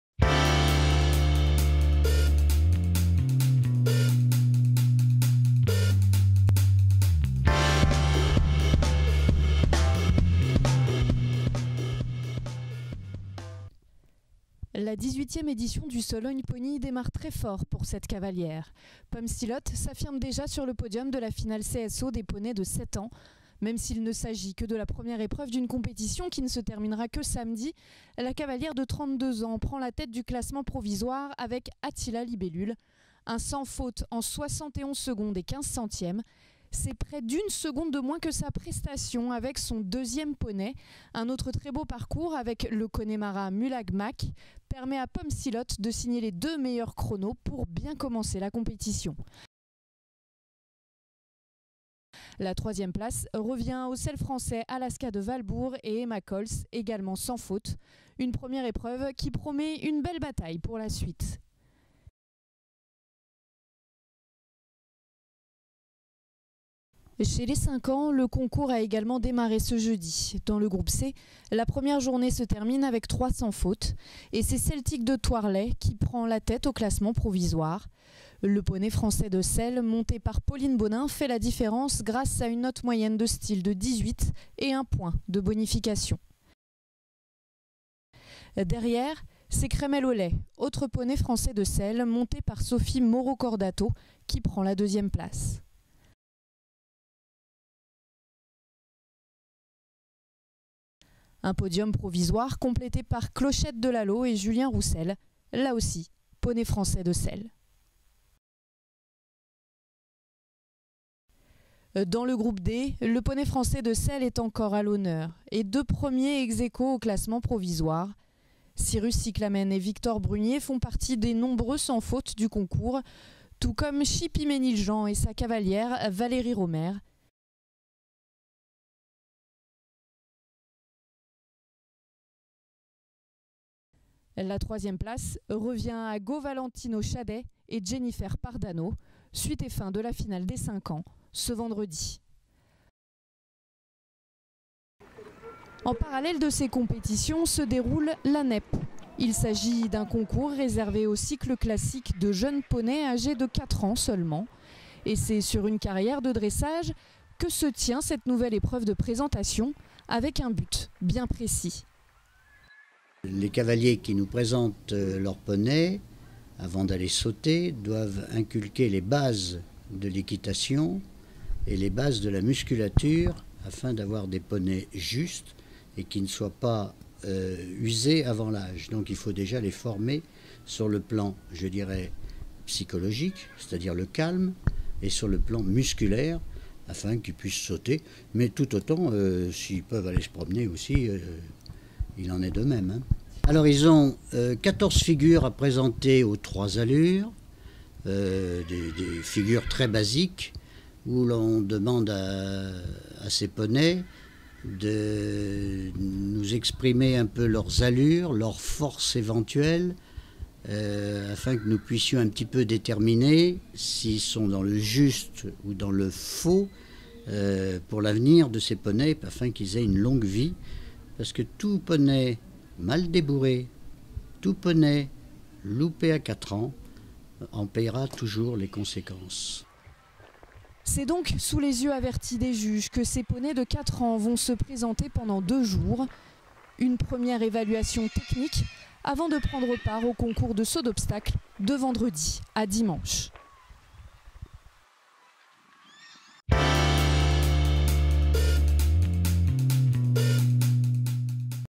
Sologn'Pony 2017 - La Quotidienne de L'Élevage - Jour 1
Revivez la première journée du Sologn'Pony, avec les épreuves 5 ans C et D, 7 ans, Nep et Saut en liberté.
François Belz, président de la commission CCE de la SHF, nous parle du jugement en épreuve d'élevage.